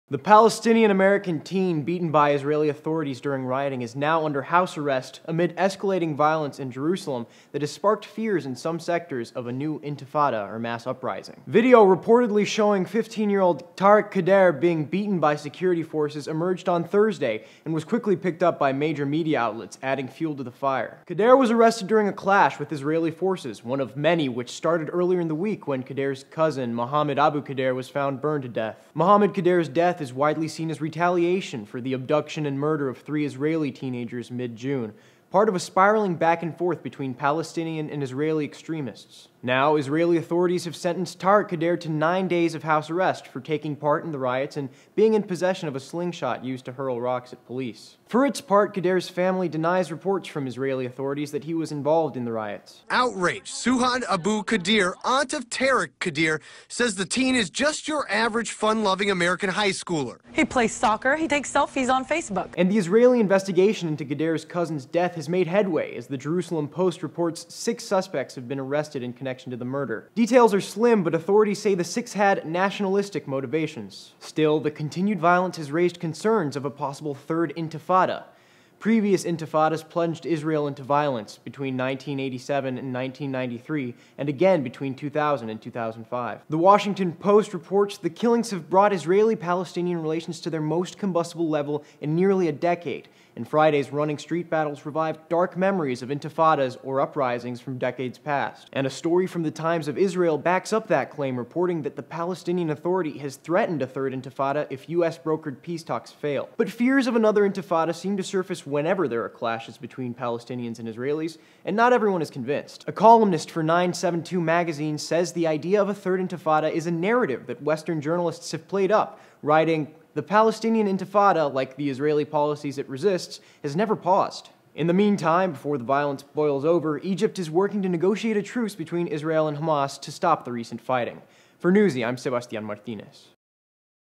Israeli-Palestinian Clashes Prompt Fears Of Third Intifada
The Palestinian-American teen who was beaten by Israeli authorities has been sentenced to house arrest.